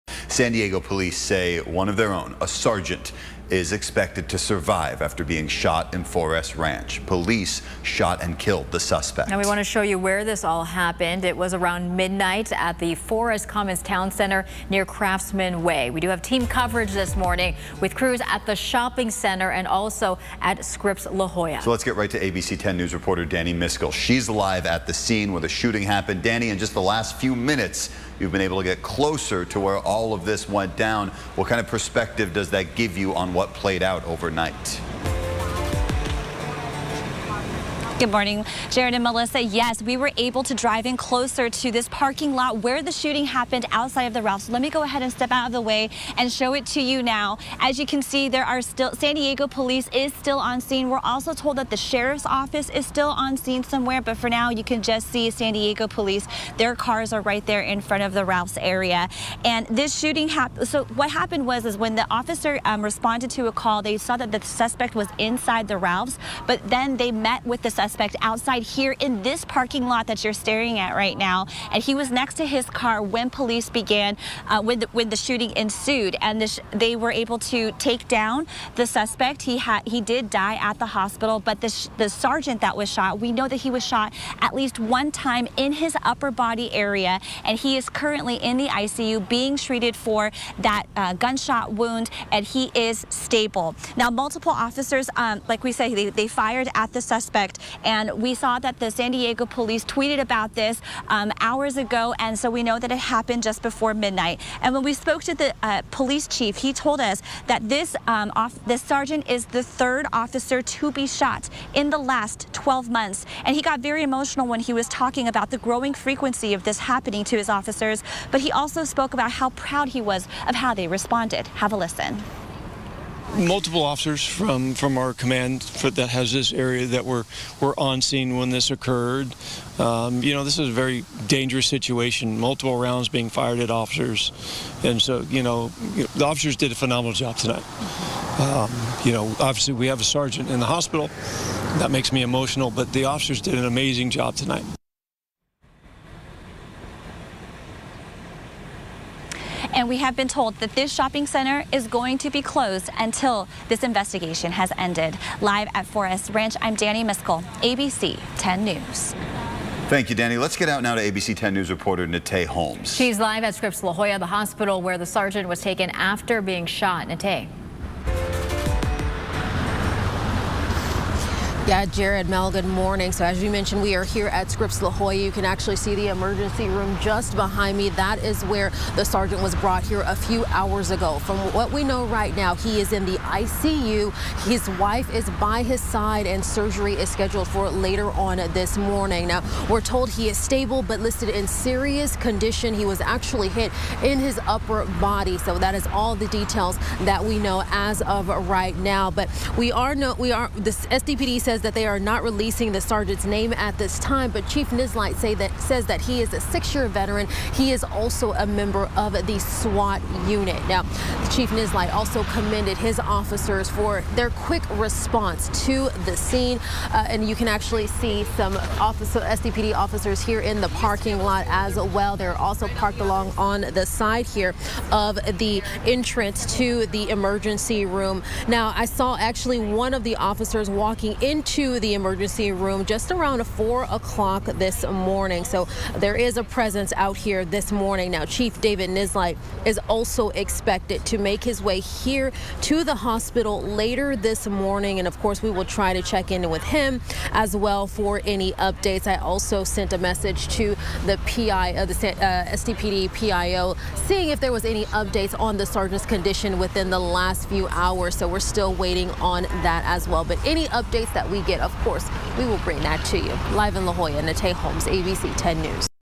Shooting in 4S Ranch leaves SDPD sergeant wounded, suspect dead
A San Diego Police sergeant was shot and a suspect was killed in a shooting that took place at a 4S Ranch shopping center late Thursday night.